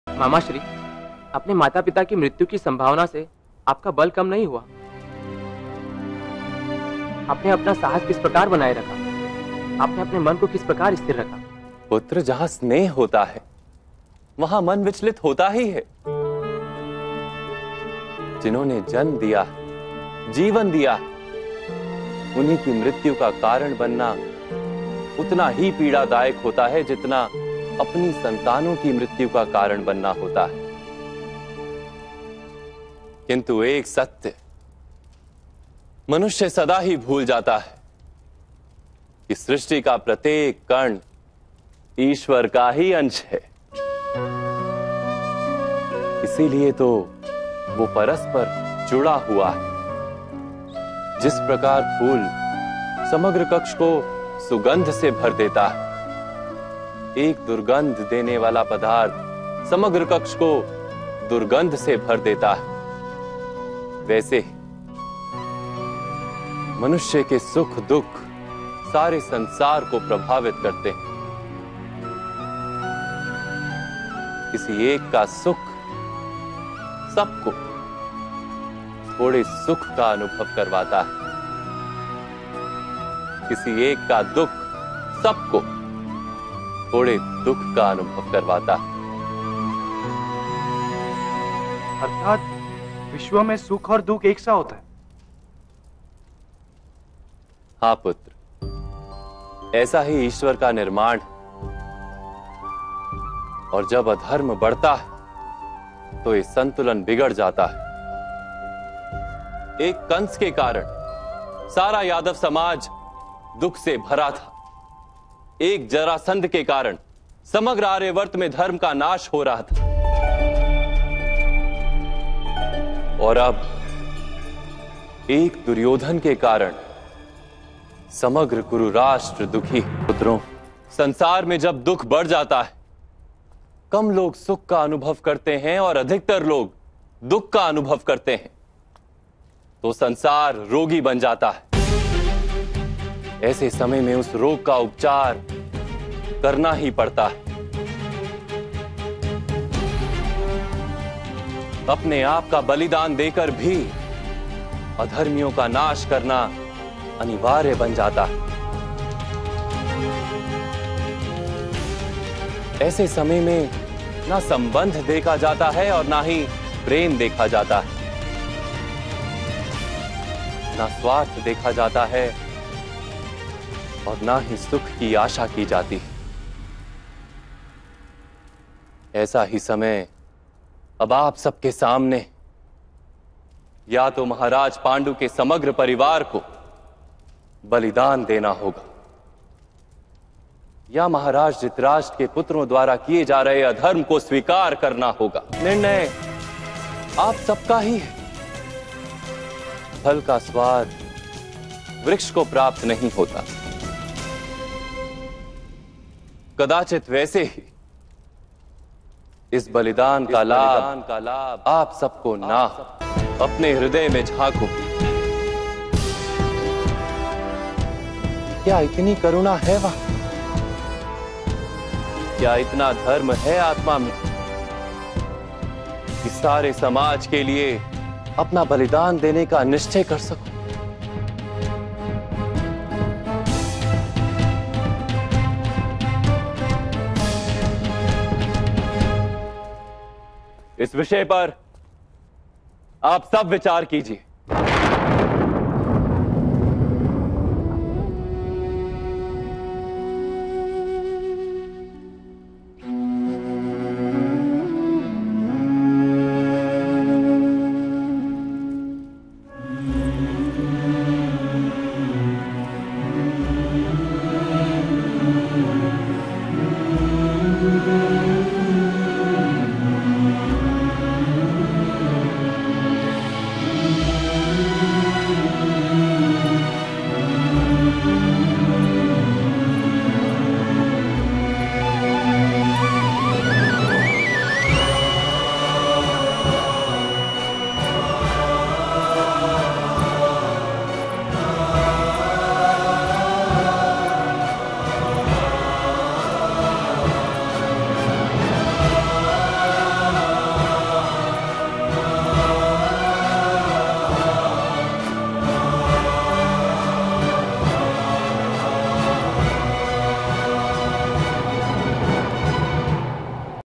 Sacrificing for the World | Krishna Gyan through Krishna Story, Part 4 (with translation)
Sacrificing for the World | Sansaar Ke Sukh Ke Liye Balidaan Dena
Plot: Kansa is Defeated by Krishna!

Krishna Gyan through Krishna Story Translation:
Prativindhya: Mamashree, the possibility of your parents' death didn't make you weak? How did you maintain your courage? How did you steady your mind?

Krishna: Son, where there is love, their mind does get distracted. The ones who gave birth gave Life, it becomes painful to be the reason for their death in the same way as to be the reason for our child's death. But one truth, a human always forgets, that every particle of this Universe is a form of the Lord only. That is why it is attached in a Globe. Like a flower which fills the entire room with fragrance, a smelly substance fills the whole room with the smell. Similarly, a human's happiness and sorrow influence the entire world. Someone's happiness gets some people to experience little joy. Someone's misery makes some people experience slight discomfort.

Shatanika: Which means in the world, happiness and sorrow are alike?

Krishna: Yes, son, this only is the Lord's creation. And when Adharma rises, then this balance gets disturbed. Because of one Kansa, the entire Yadava society was in sorrow. Because of one Jarasandha, Dharma was getting destroyed in the whole Aryavarta. And now because of one Duryodhan, the Kururashtra as a whole is unhappy.

Sons, in the world when the sorrow increases, fewer people experience happiness, and more people experience sadness. Then the world becomes a patient. At this time, that disease should be cured. It becomes necessary to sacrifice yourself and to destroy Adharma people. In this time,  neither relationships are seen, nor love. Neither selfishness is seen, nor happiness is expected. The same time is in front of you all, either the King Pandu's entire family will have to sacrifice or will have to accept the Adharma done by the sons of Dhritarashtra.

The tree doesn't get the taste of the fruit. Perhaps, in the same way, you all might not benefit from the sacrifice. Take a peek in your Heart, is there enough compassion? Is there enough Dharma in the soul? Deciding to sacrifice for the entire society? Aap Sabhi Swayam Vichaar Kijiyega You all think upon this Topic.

(Draupadi leaves the room)